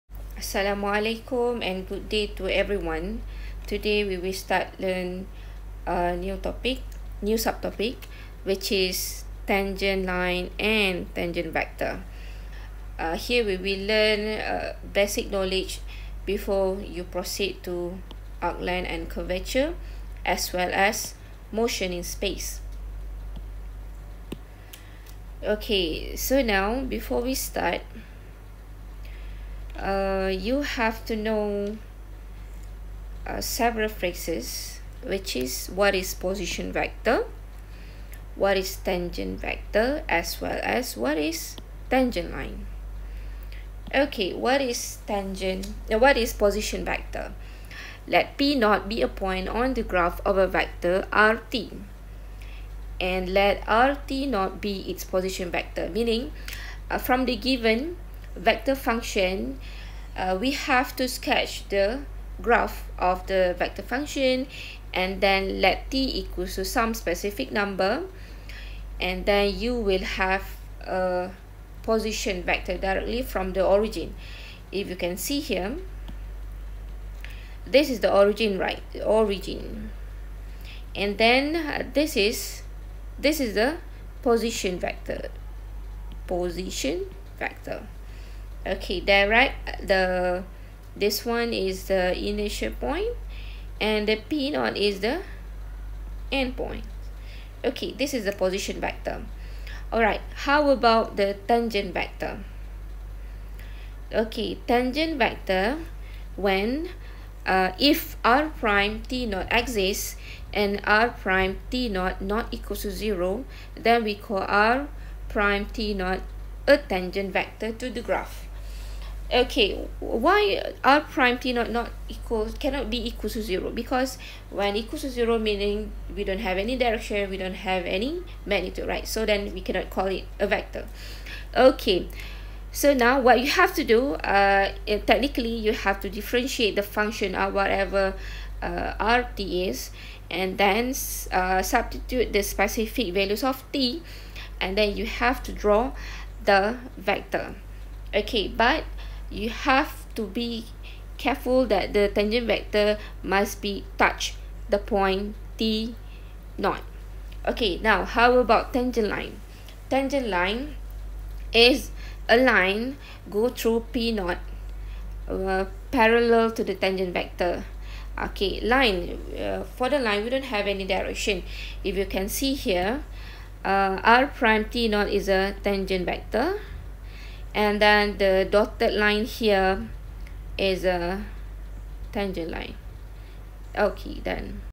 2 2 2 A
APPLIED CALCULUS. 2.2.2 (A) Derivatives and Integrals of Vector Function: Tangent Line and Tangent Vector